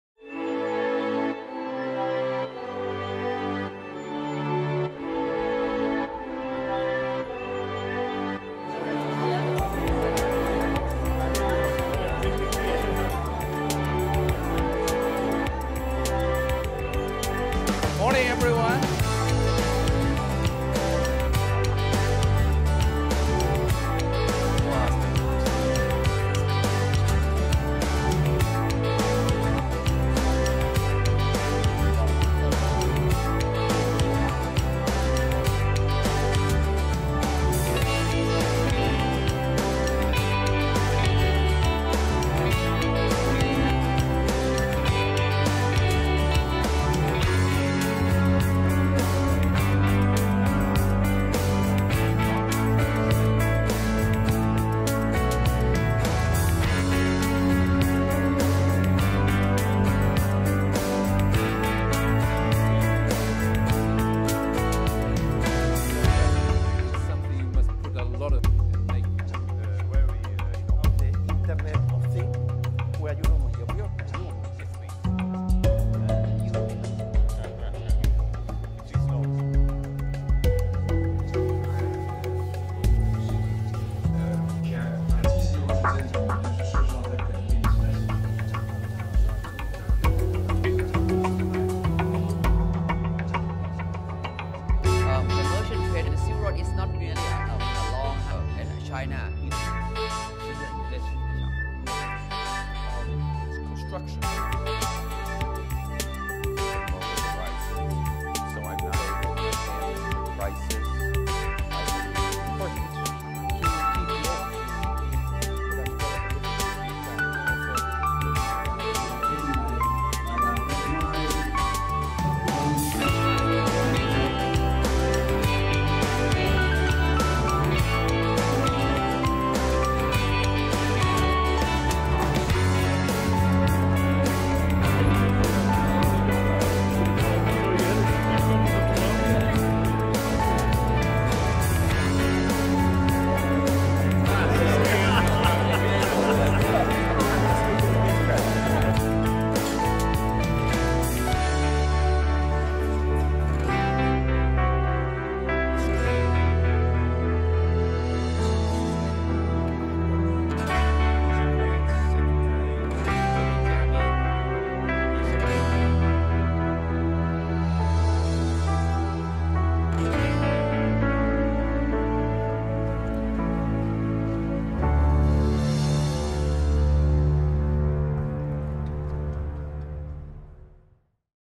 Horasis Global China Business Meeting 2013 - Highlights
The 2013 Horasis Global China Business Meeting took place in The Hague, co-hosted by NCH.